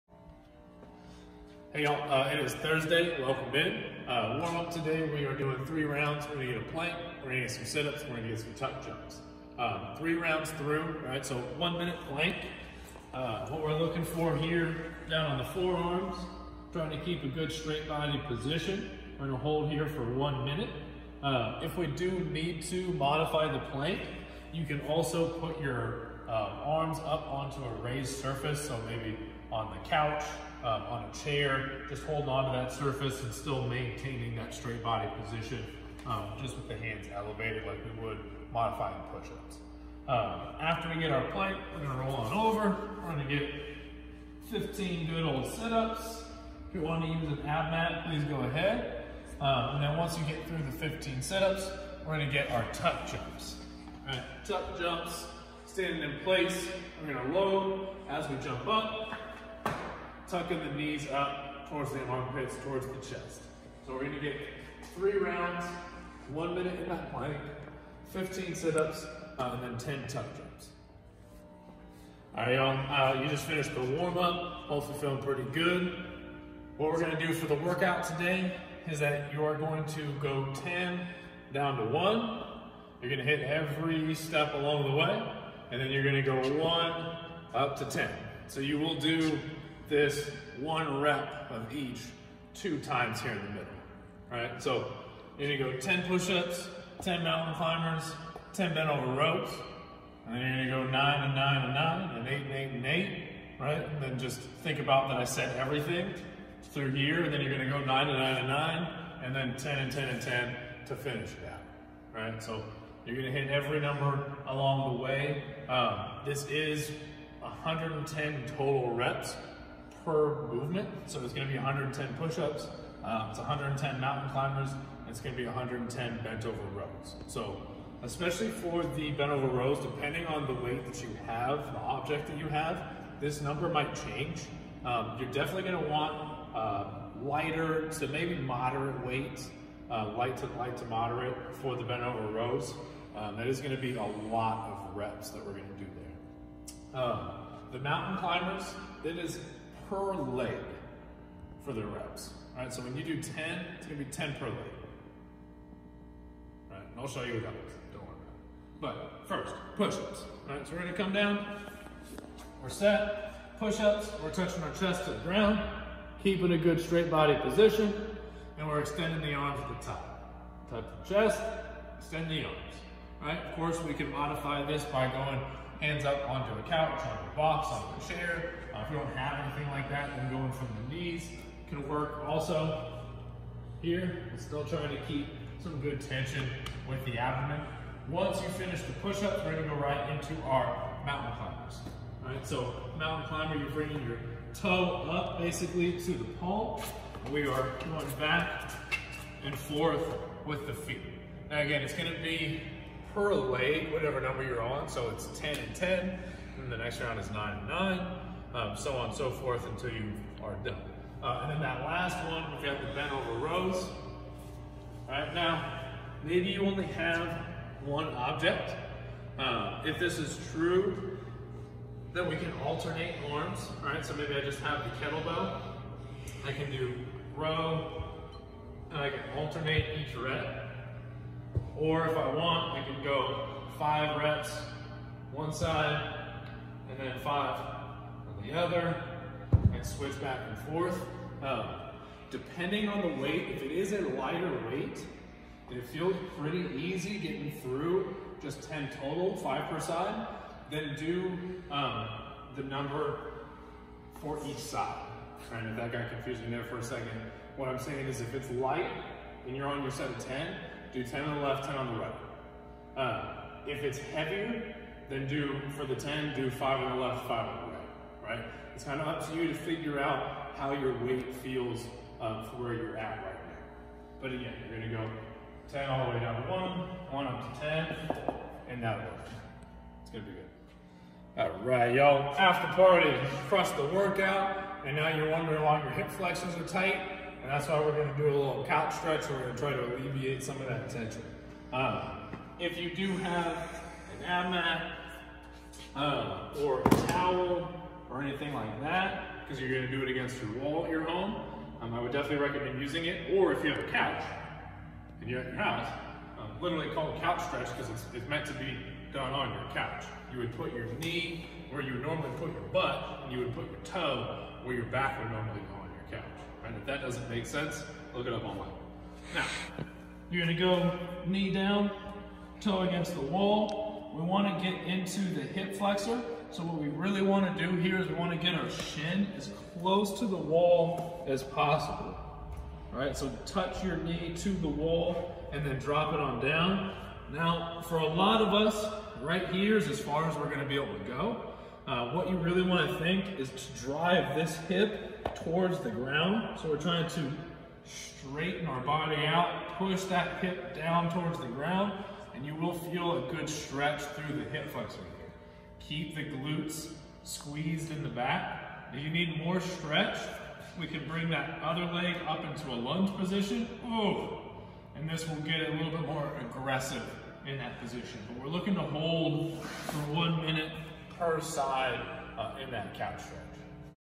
CrossFit 404 - Home WOD#42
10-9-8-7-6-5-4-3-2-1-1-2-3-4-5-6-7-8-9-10
10 down to 1, then 1 up to 10
Push-Ups
Mt. Climbers
Bent Over Rows